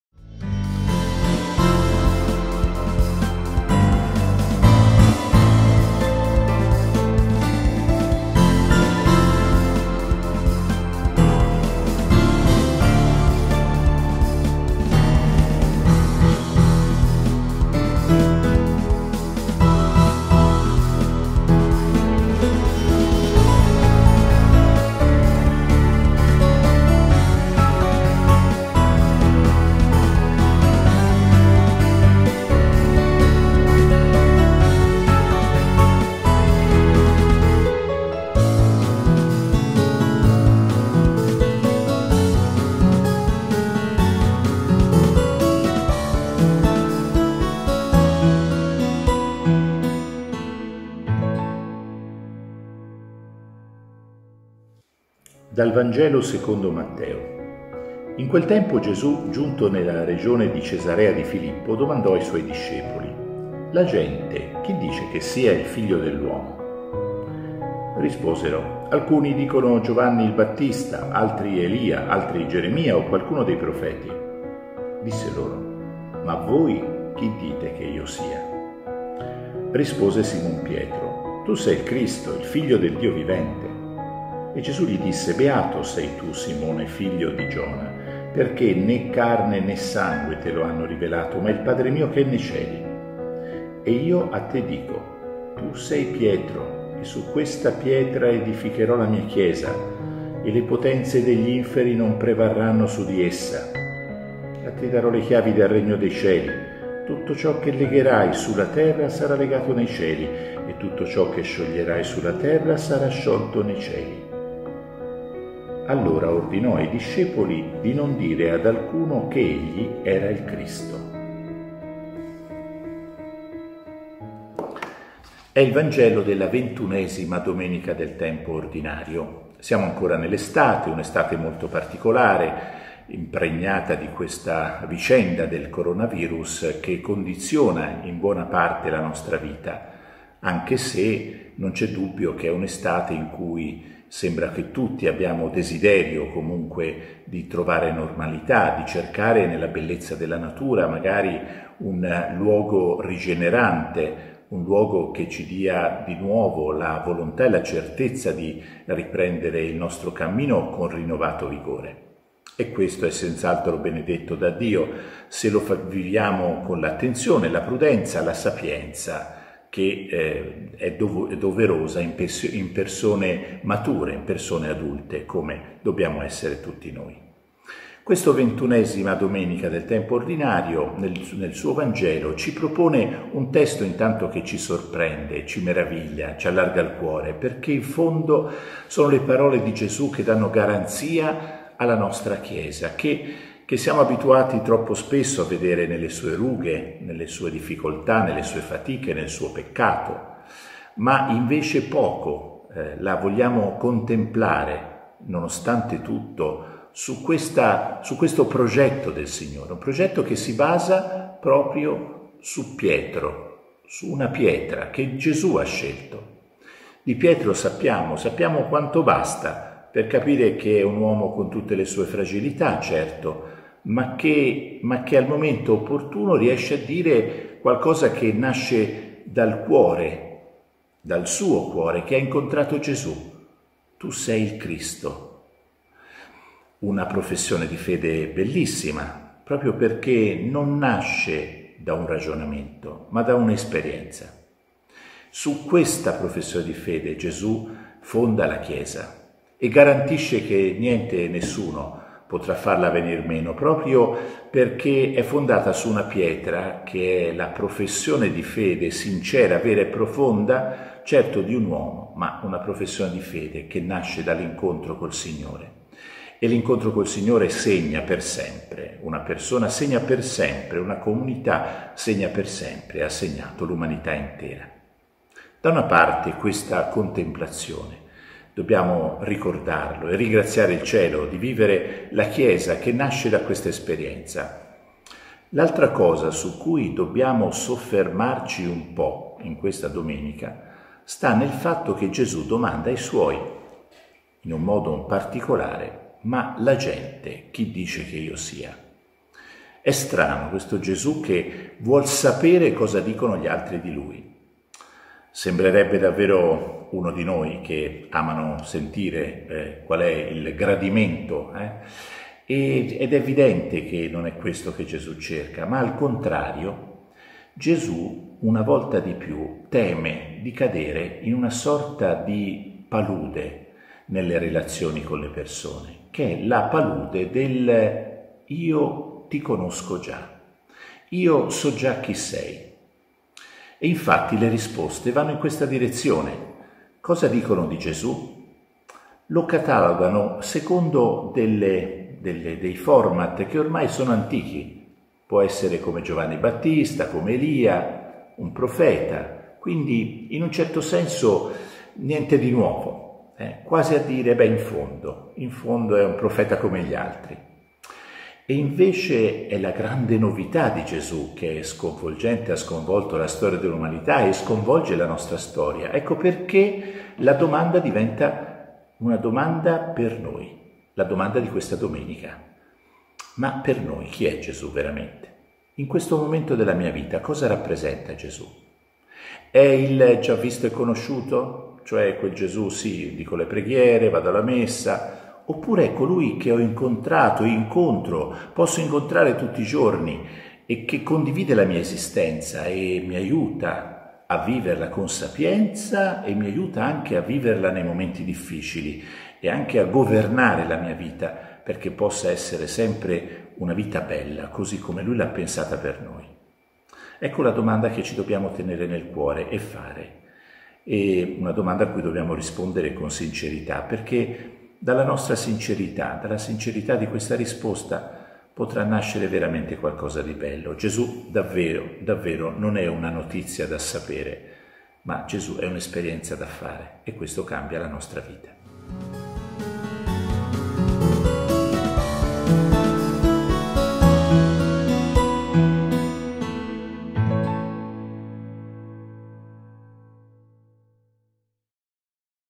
Fermati ancora un po' XXI Domenica del tempo ordinario
Mons. Maniago, Vescovo di Castellaneta: "Il Signore vuole tirarci fuori dalla palude che spesso fa affondare le relazioni, anche quella con Lui: dare tutto per scontato, affermare "ma io ti conosco già", chiudendosi allo stupore della novità di Dio".
In ascolto del Vangelo della XXI Domenica del Tempo Ordinario.